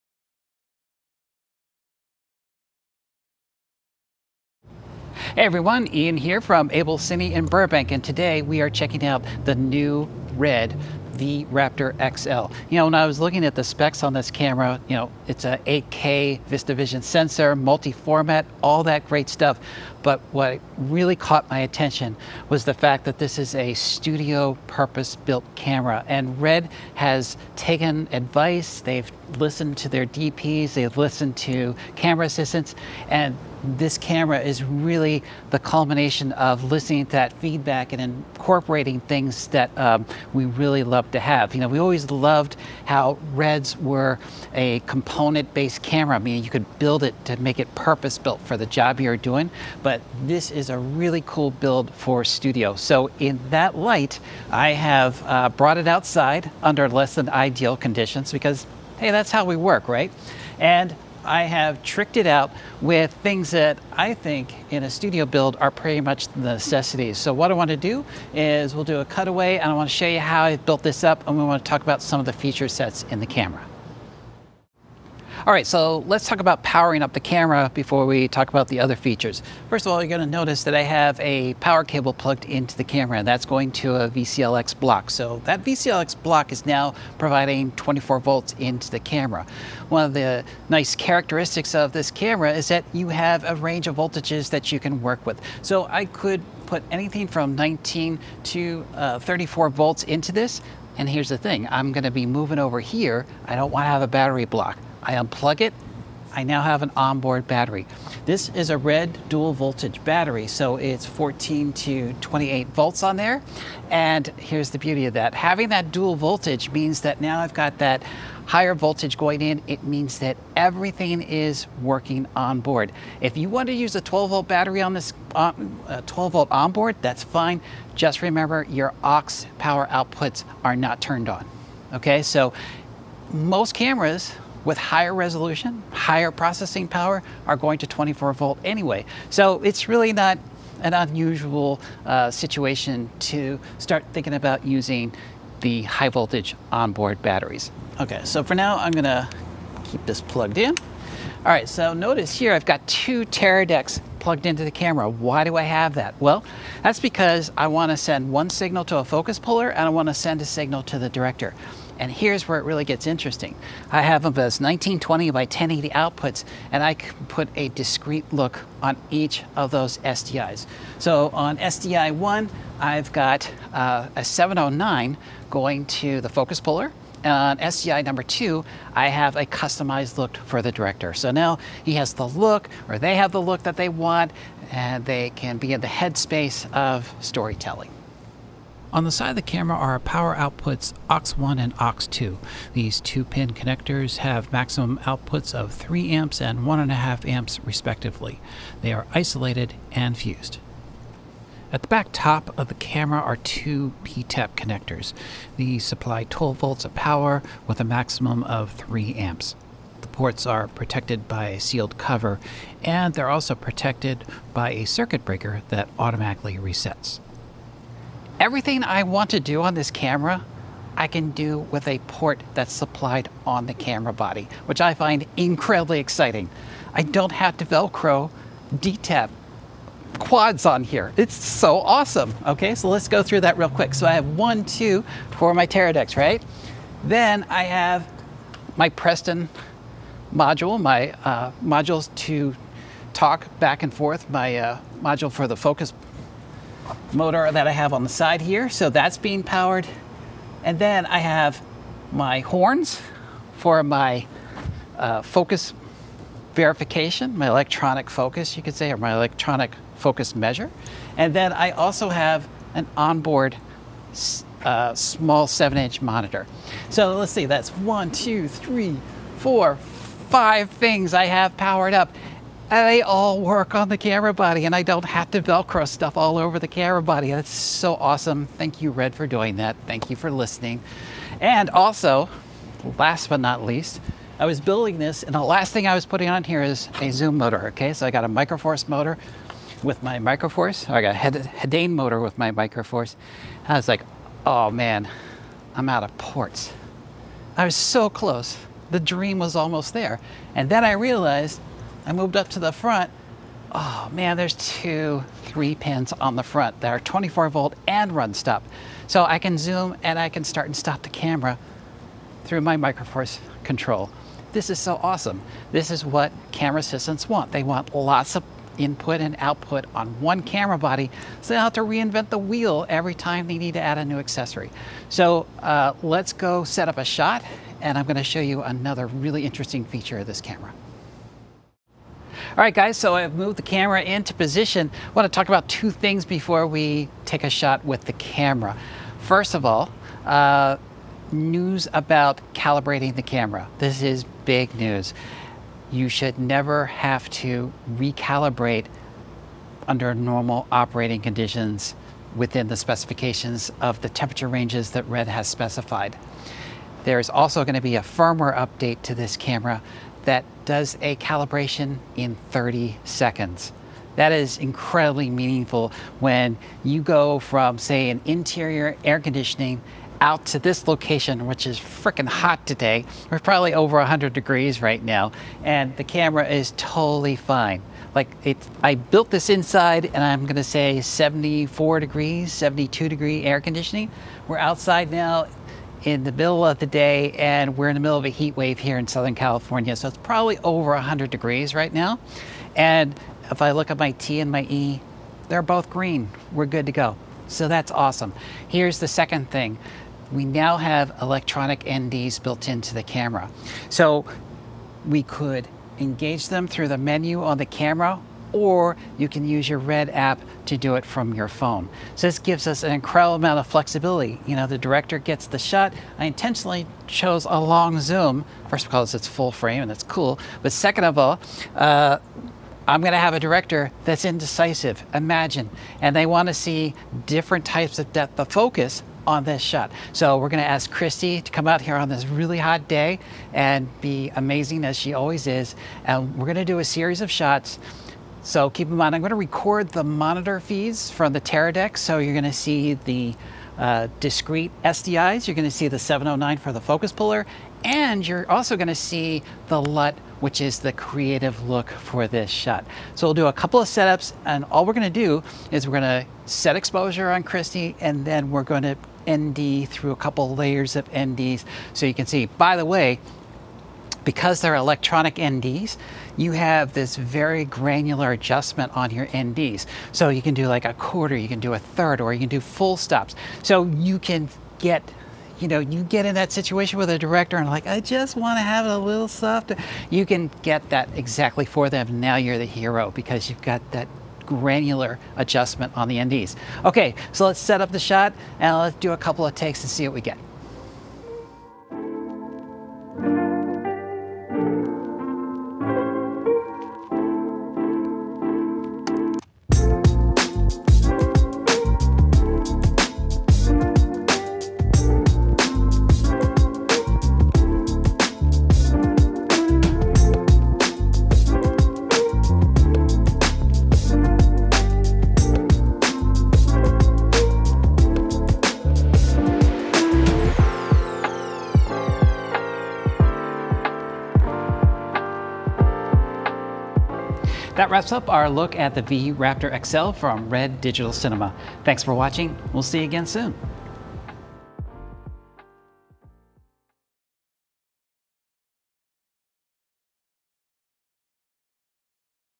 A Closer Look at the RED V-RAPTOR XL
Ian takes a look at the RED V-RAPTOR XL, RED's new purpose-built studio camera. This new flagship camera takes everything you love about RED and adds new features that DPs and camera assistants have been requesting.

Register for our virtual RED V-RAPTOR XL Quick Start class